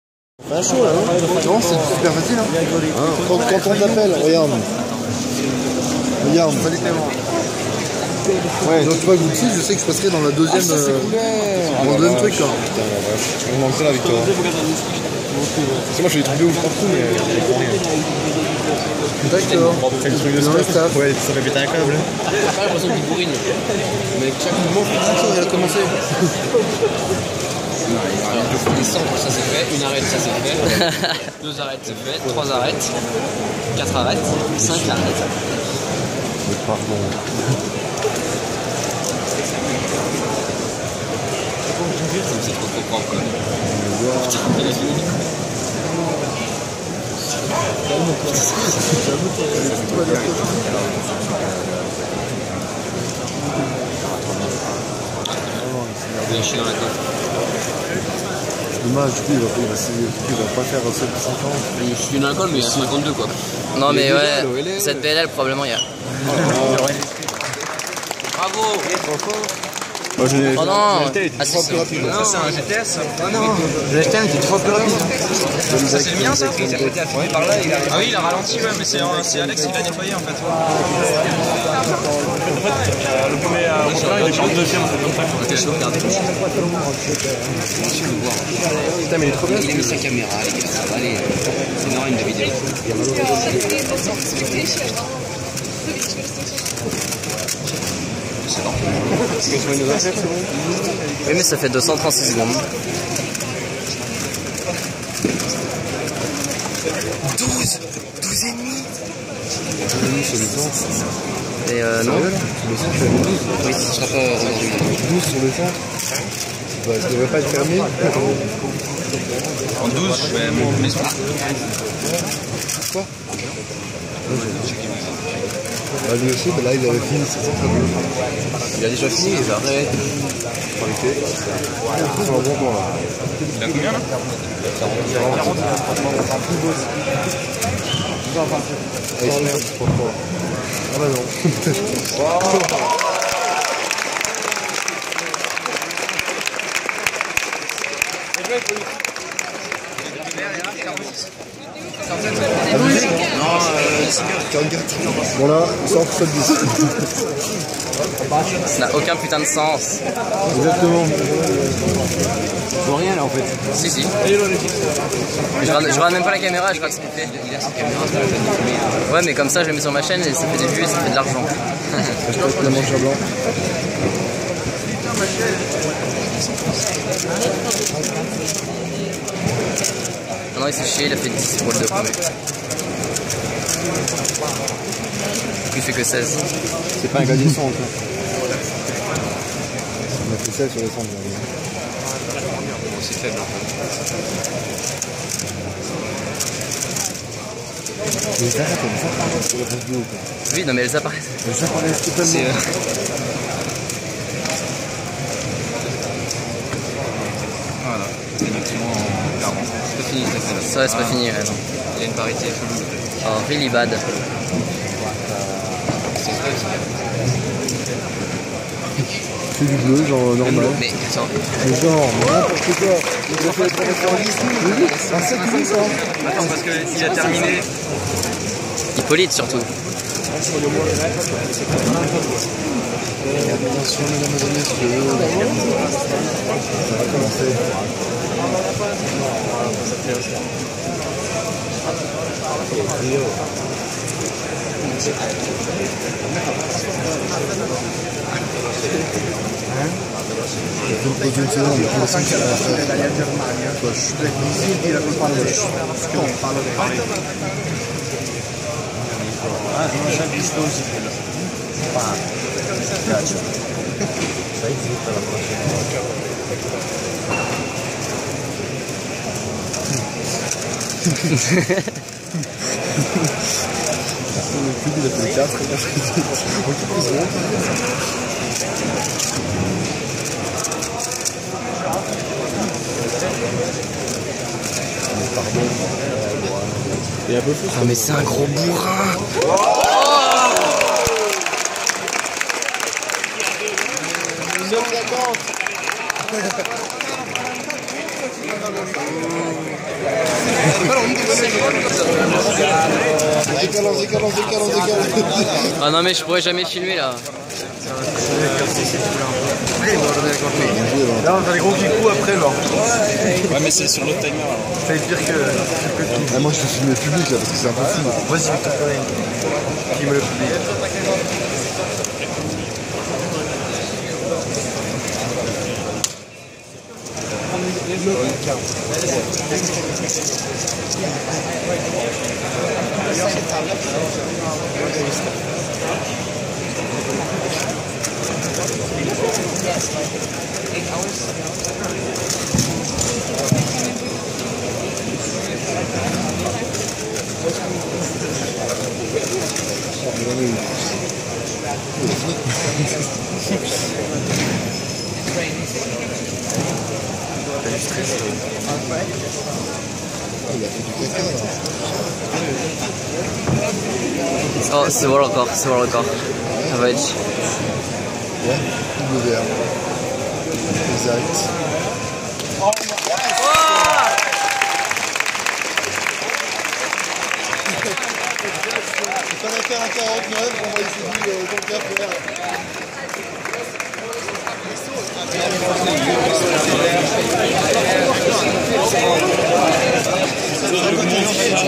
5x5 cube world record average: 49.32 Feliks Zemdegs
1st round of European Championships in Prague